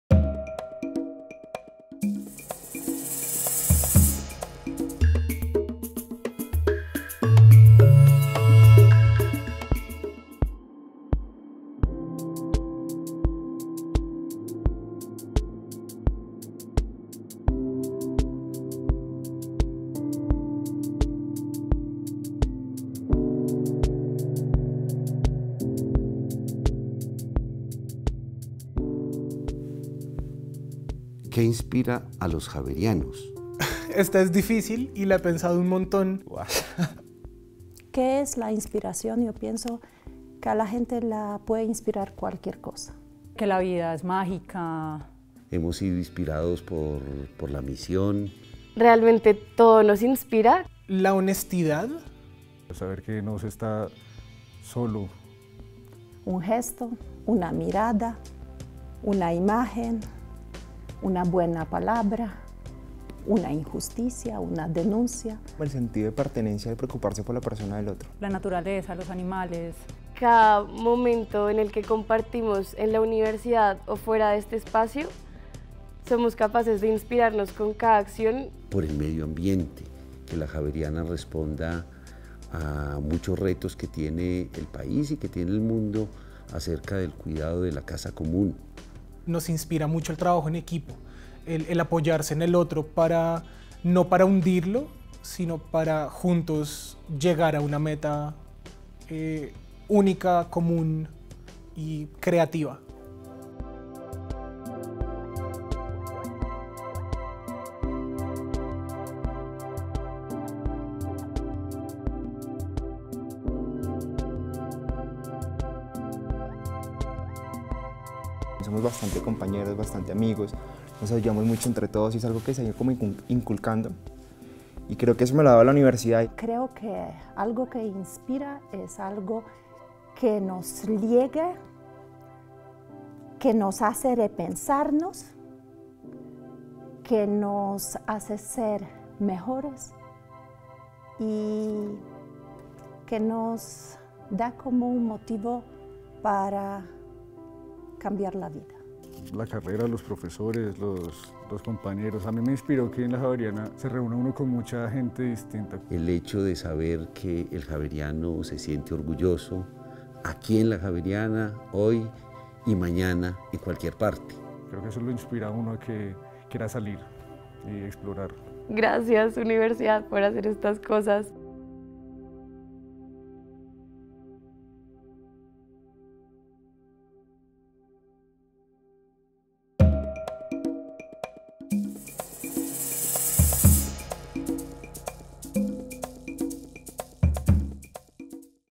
Editorial #20 ¿Qué los inspira?
En esta oportunidad quisimos innovar la sección editorial con un nuevo formato audiovisual. Fue así como reunimos a profesores, administrativos y estudiantes para que fueran parte de este proyecto. En estos tres minutos, podrán ver a 7 personajes de la comunidad javeriana que nos respondieron: ¿Qué los inspira?

En sus respuestas nos contaron una historia acerca de ellos mismos y la comunidad Javeriana.